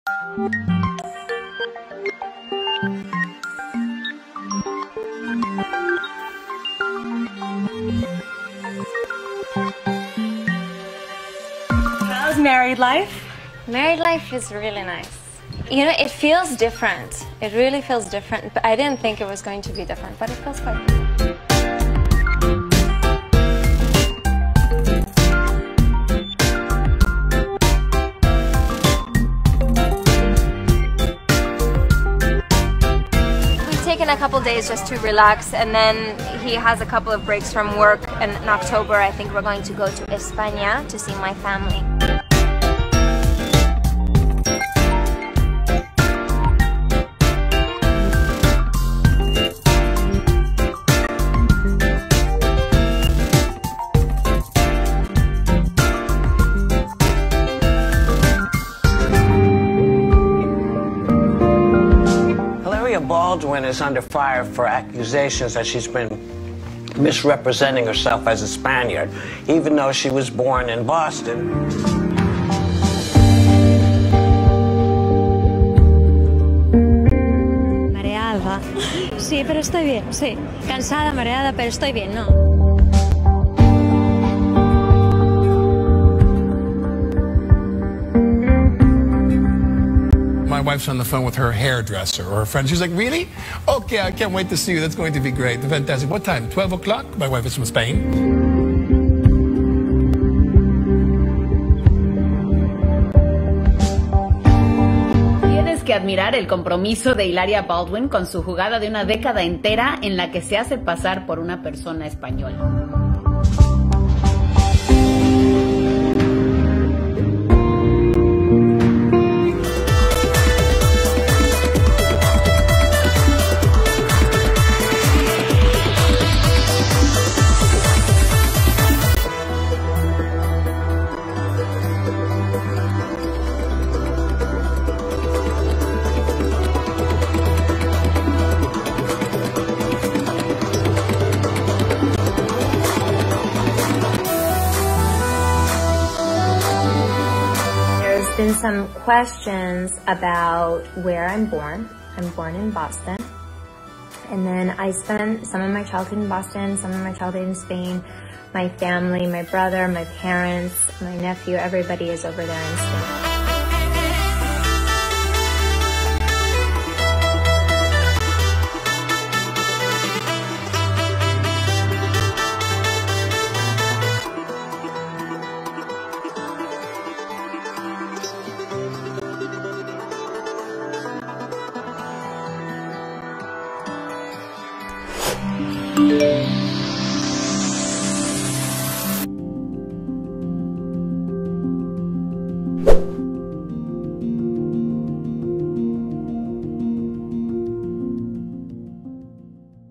She pretended to be Spanish for a decade | Positive
Hilaria Baldin became known in 2012 as Alec Baldwin's wife. She implied on several occasions that she was Spanish. She faked a strange accent, and her image was quite "Hispanic". No one could have ever guessed that it was all a strange lie. A decade later, her best kept secret would be revealed. Her name was Hilary, and her birthplace was Boston, Massachusetts. Her lie fell apart and she could do nothing to stop it.